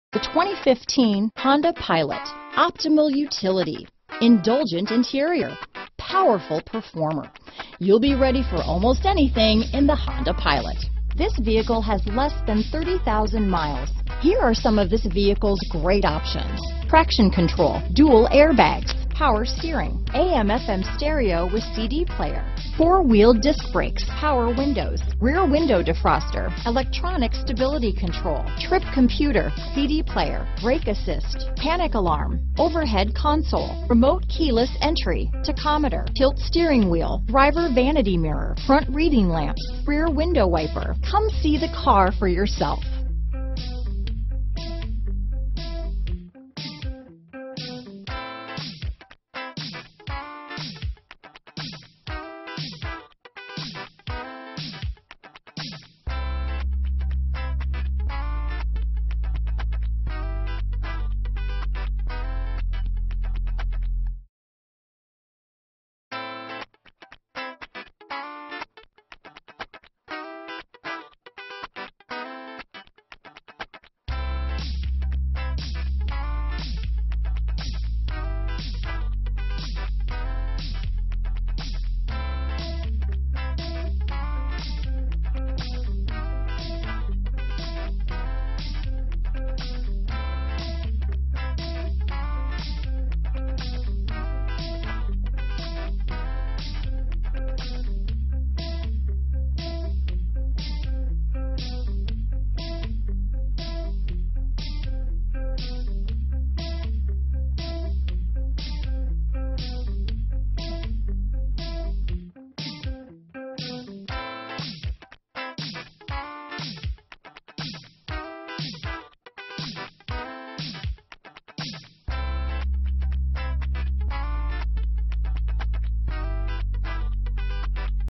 2015 Honda Pilot Schaumburg, Arlington Heights, Hoffman Estates, Des Plaines, Palatine, IL 86177A
2015 Honda Pilot
Schaumburg Honda: Servicing Schaumburg, Illinois near Arlington Heights, Hoffman Estates, Des Plaines, and Palatine, IL
2015 Honda Pilot  - Stock#: 86177A - VIN#: 5FNYF4H29FB062490

Schaumburg Honda
750 E Golf Rd

Recent Arrival! CARFAX One-Owner. 2015 Honda 4WD Pilot LX Obsidian Blue 5-Speed Automatic Clean CARFAX. Odometer is 15545 miles below market average!